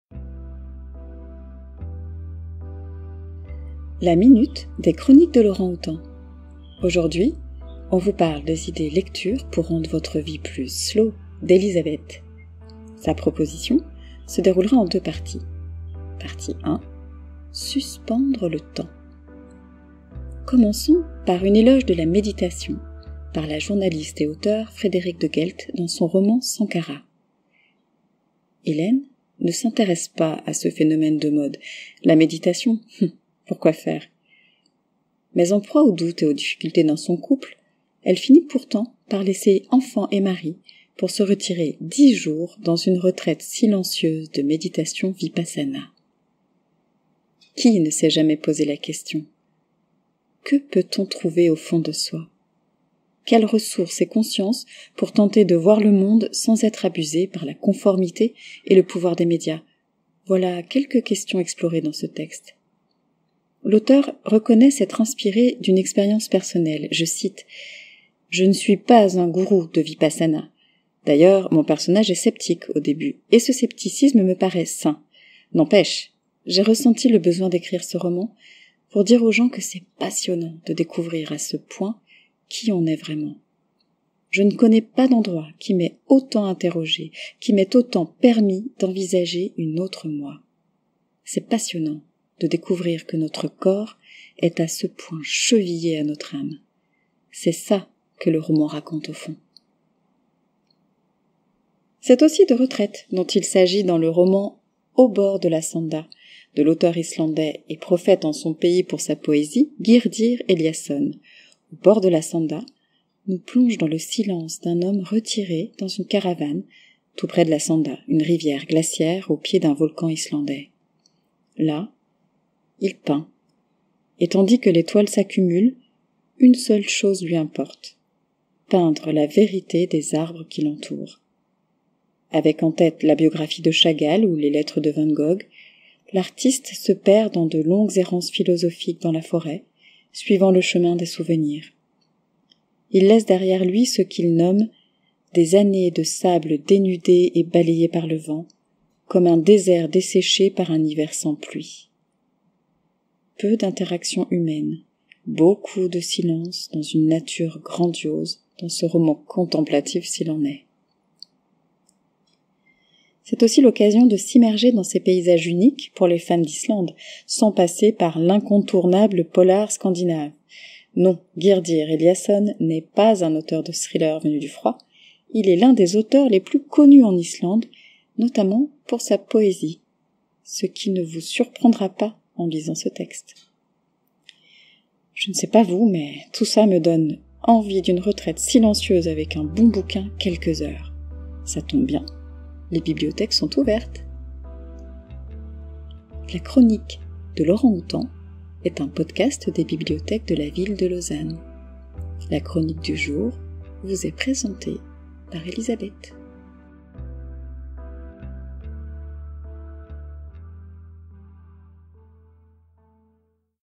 [S L O W] Ralentir et suspendre le temps | Podcast #53
Et si on ralentissait un peu?
Voici une sélection de romans de la part d'Elisabeth pour ralentir le temps. Première partie.

Les œuvres citées:
Sankhara, de Frédérique Deghelt, Actes Sud, 2020.
Au bord de la Sanda, de Gyrdir Eliasson, La Peuplade, 2019.